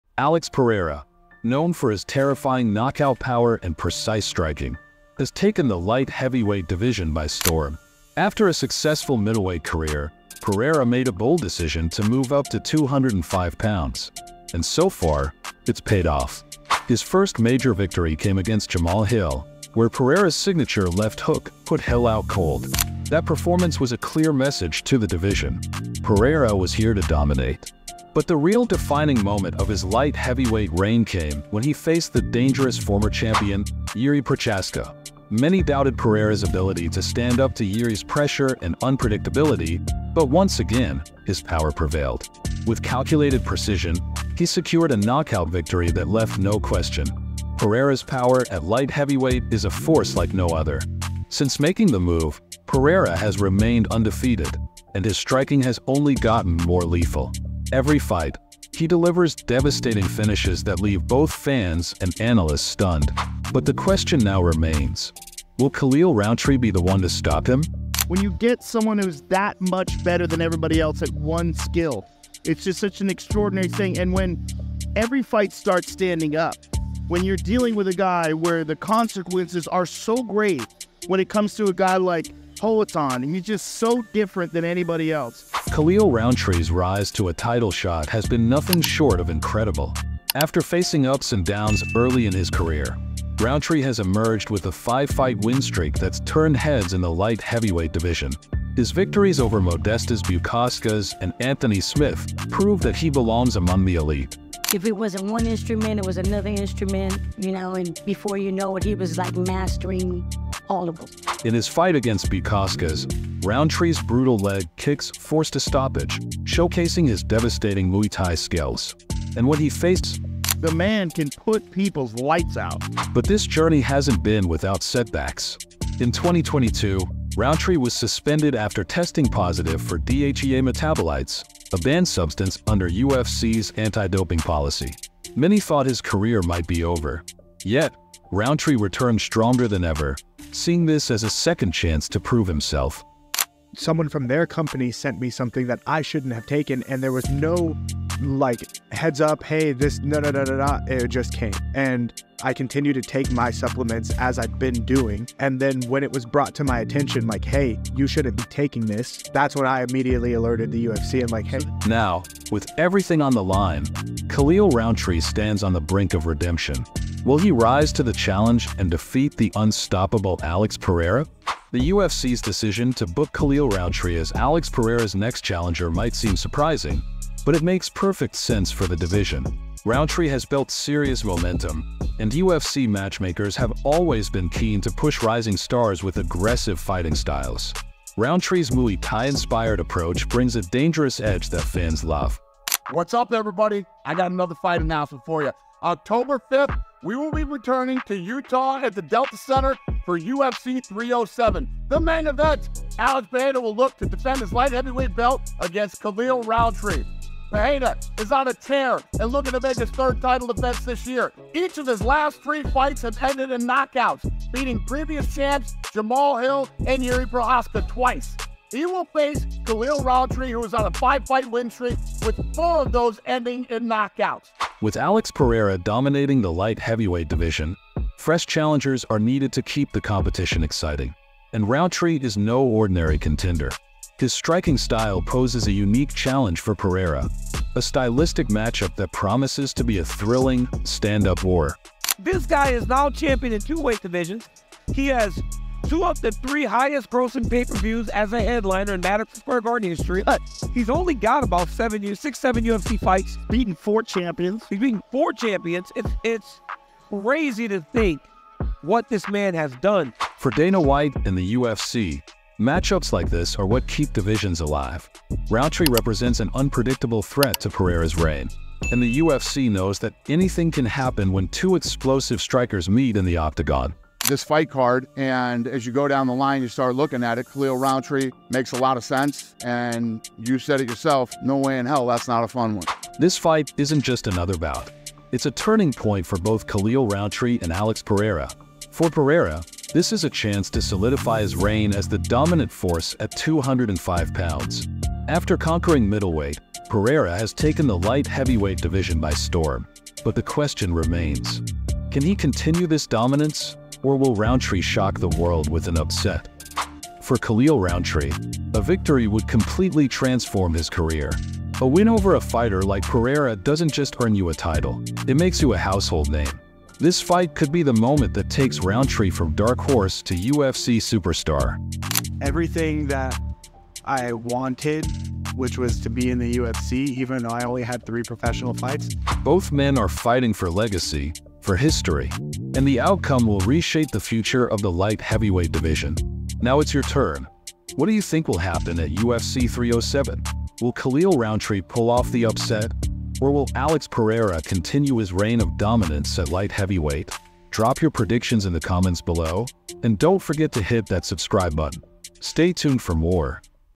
The Fight No One Saw Coming | Khalil Rountree vs. Alex Pereira at UFC 307
What led to the most surprising matchup at UFC 307? Khalil Rountree vs. Alex Pereira is a fight no one saw coming. In this breakdown, we reveal the real reason behind this unexpected booking and what each fighter has on the line. Will Rountree rise to the occasion, or is Pereira on the verge of another highlight-reel knockout? Find out now, and drop your predictions in the comments below!